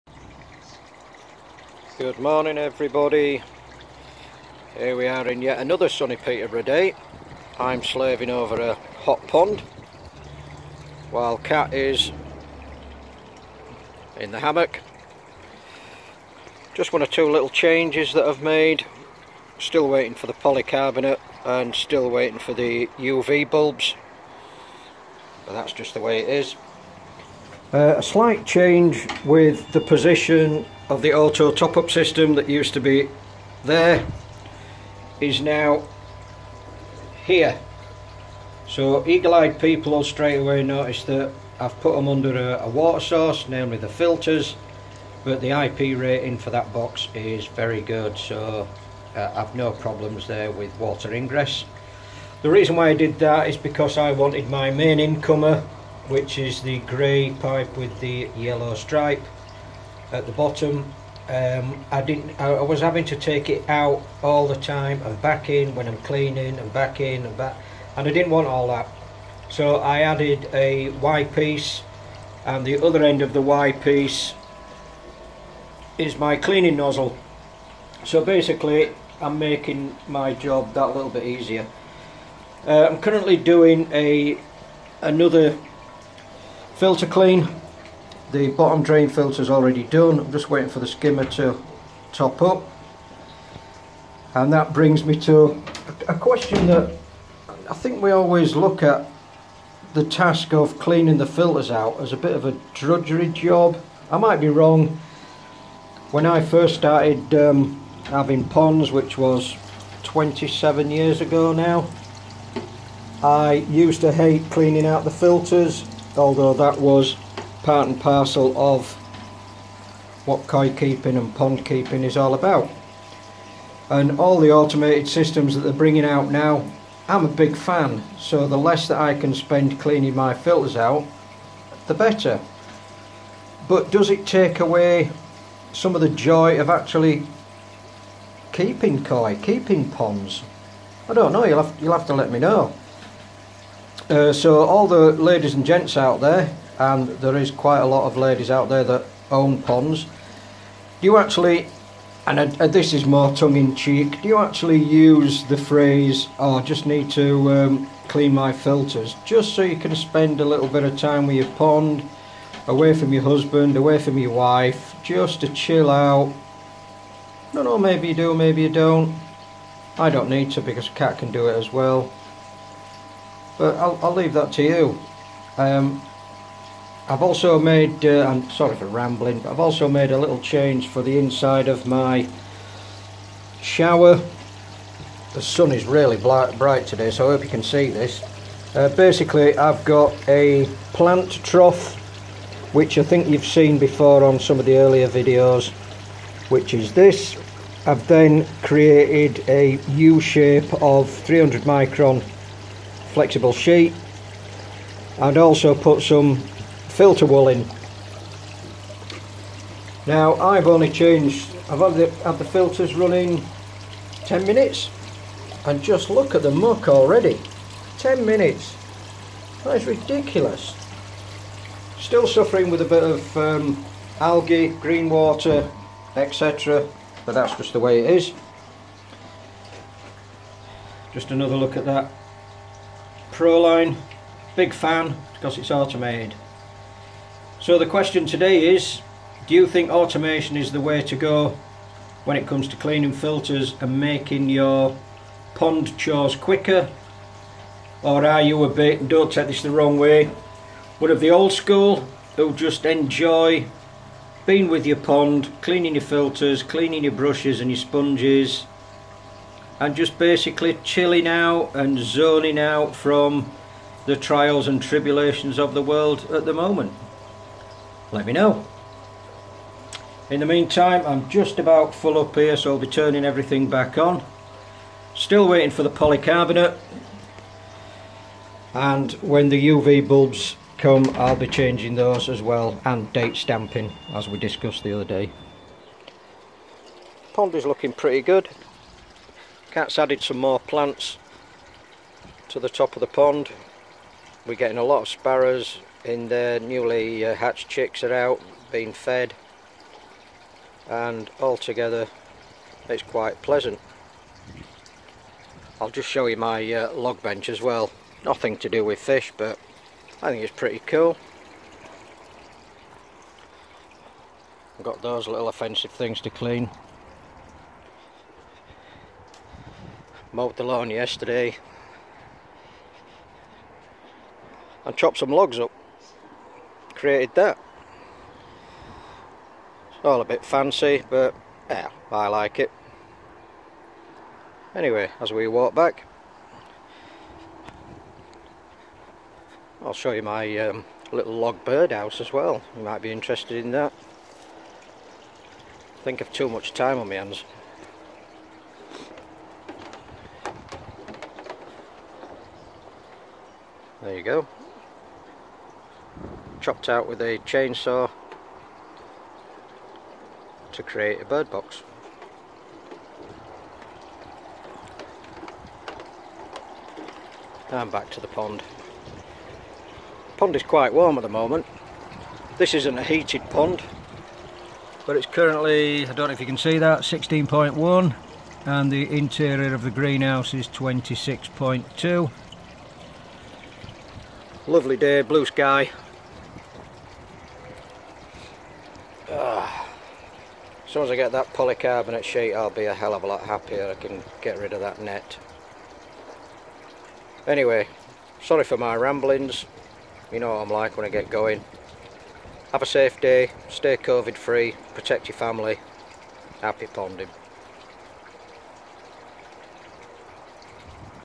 Modifications, mumblings and modern maintenance.
Moved the auto top up, added a Y piece for quicker cleaning. Added a 300 micron screen and filter wool in shower and are you happy to clean filters or would you prefer automated systems. My subscriber list is climbing and I thank you all for that.